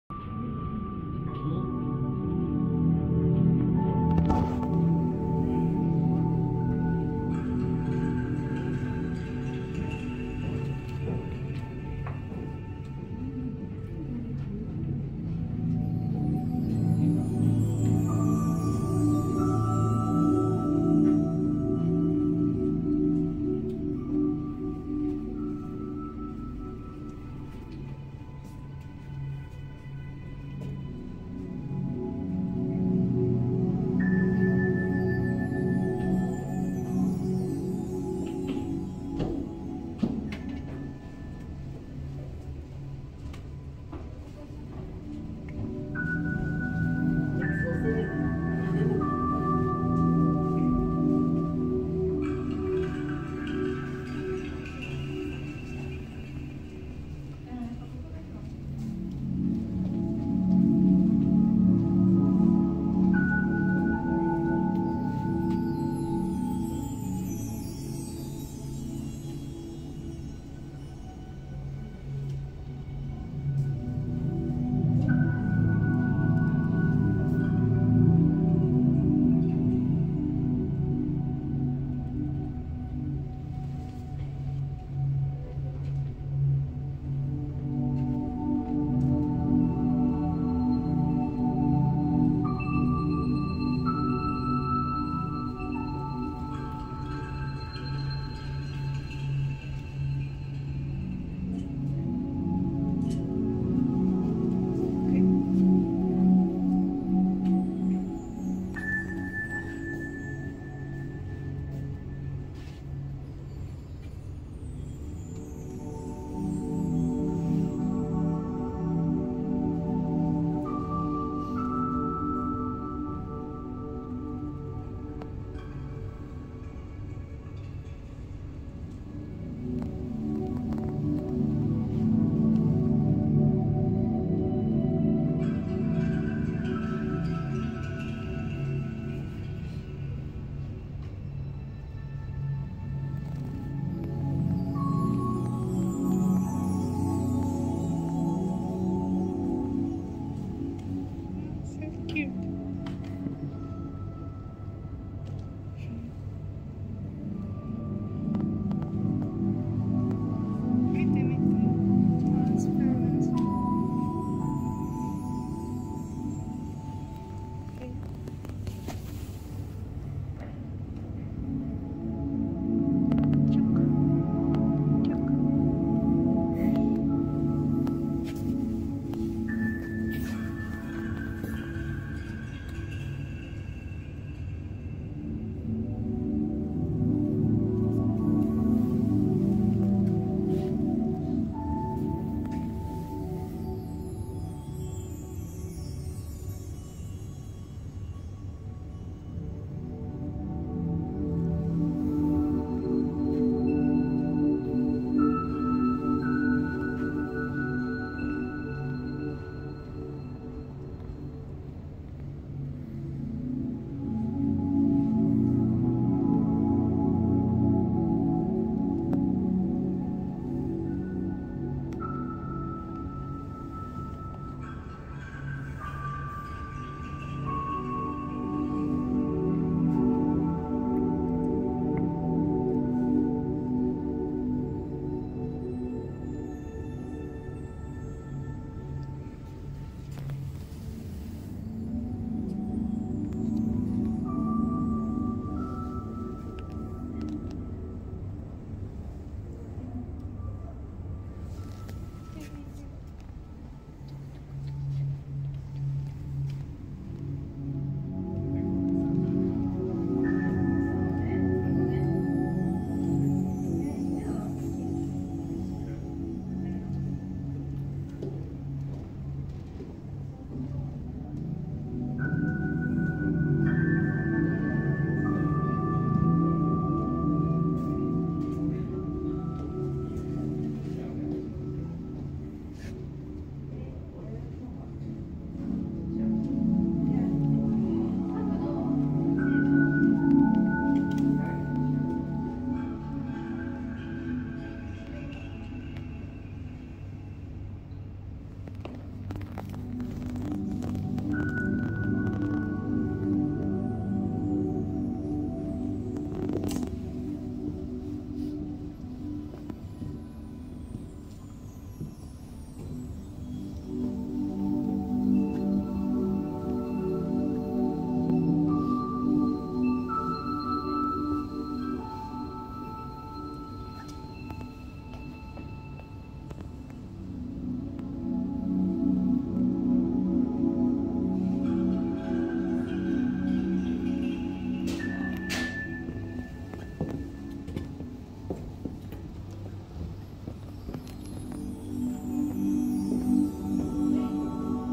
Tokyo's Ocean Oasis: A Tour of Skytree Aquarium
Dive into the mesmerizing world of Skytree Aquarium with this immersive video experience. Join us as we explore the diverse marine life and captivating exhibits that await within Tokyo's iconic Skytree Tower. From colorful coral reefs to majestic ocean predators, this video offers a glimpse into the beauty and wonder of underwater ecosystems. Get up close with exotic sea creatures and embark on a virtual journey through the depths of the ocean without leaving the city. Join us as we unravel the mysteries of the deep at Skytree Aquarium.